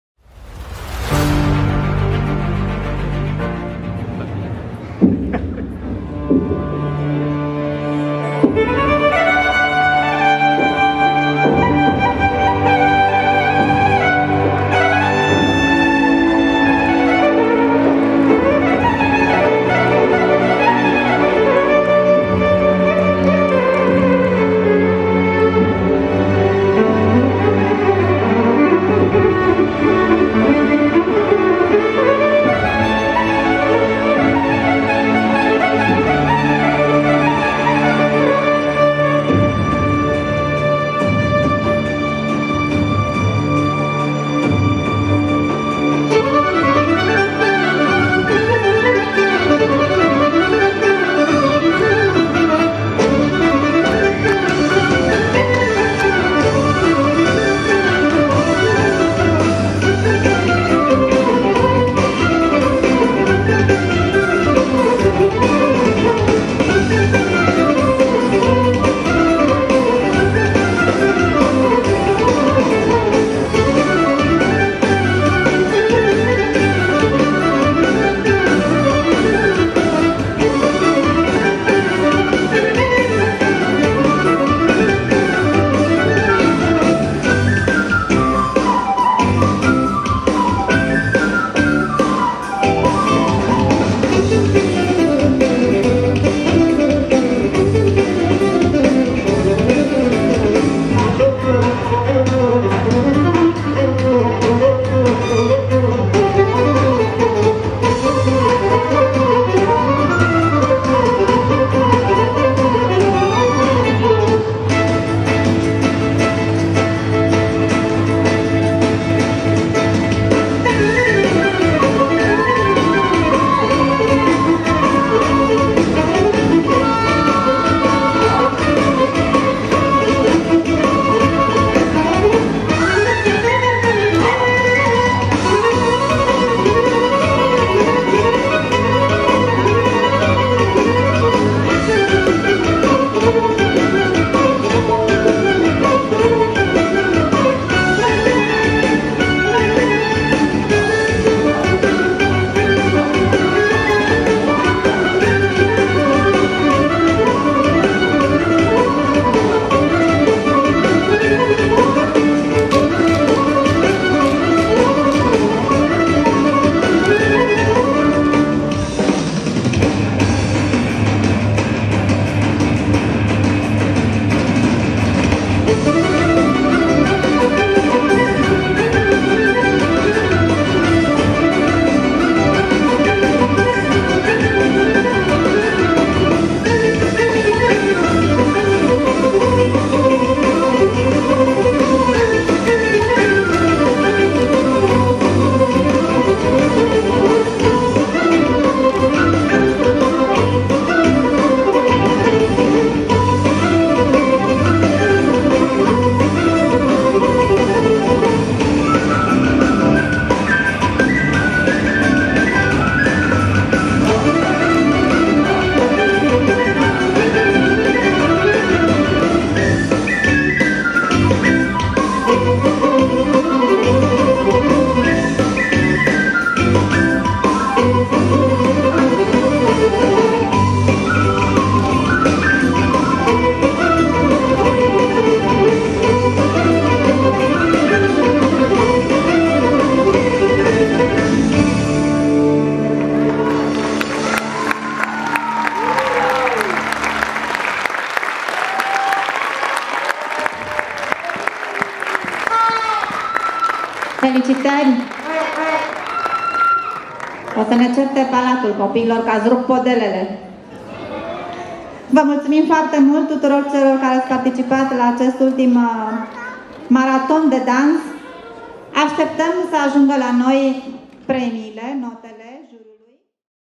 GOLDEN TIME TALENT | 47 Season | Ensemble of modern Impuls | Folk choreography
Member/team name: Ensemble of modern Impuls

The country: Moldova | City: Ocnita

Nomination: Folk choreography

Name of competitive work: Ethno composition

Educator/s: Anna Suslina

Participation in other competitions and festivals / creative achievements: Laureates and prize-winners, 1,2 and Grand Prix of International competitions in Poland, Hungary, Romania, Moldova